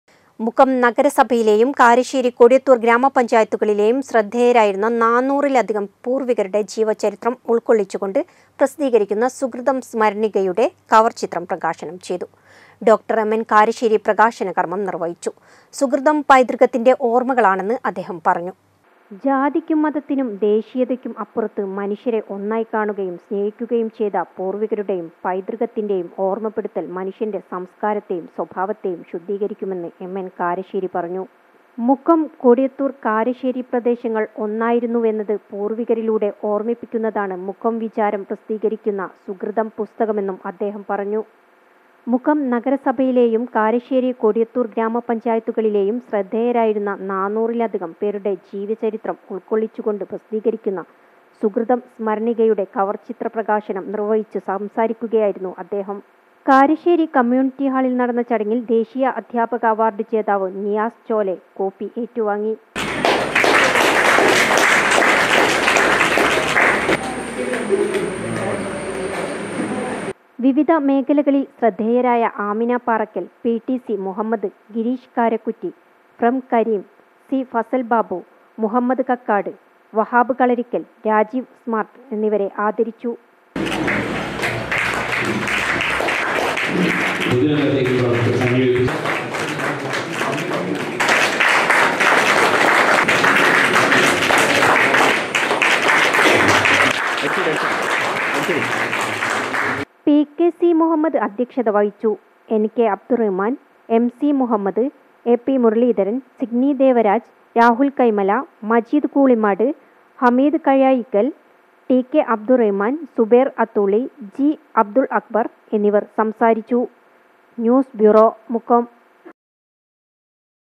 സുകൃതം സ്മരണികയുടെ കവർ ചിത്രം പ്രകാശനം ചെയ്തു. ഡോ; എം എൻ കാരശ്ശേരി പ്രകാശന കർമം നിർവഹിച്ചു.
മുക്കം നഗരസഭയിലെയും കാരശ്ശേരി, കൊടിയത്തൂർ ഗ്രാമപഞ്ചായത്തുകളിലെയും  ശ്രദ്ധേയരായിരുന്ന 400 ലധികം പൂർവ്വികരുടെ ജീവചരിത്രം ഉൾക്കൊള്ളിച്ചുകൊണ്ട്  പ്രസിദ്ധീകരിക്കുന്ന സുകൃതം സ്മരണികയുടെ കവർ ചിത്രം പ്രകാശനം ചെയ്തു. ഡോ; എം എൻ കാരശ്ശേരി പ്രകാശന കർമം നിർവഹിച്ചു.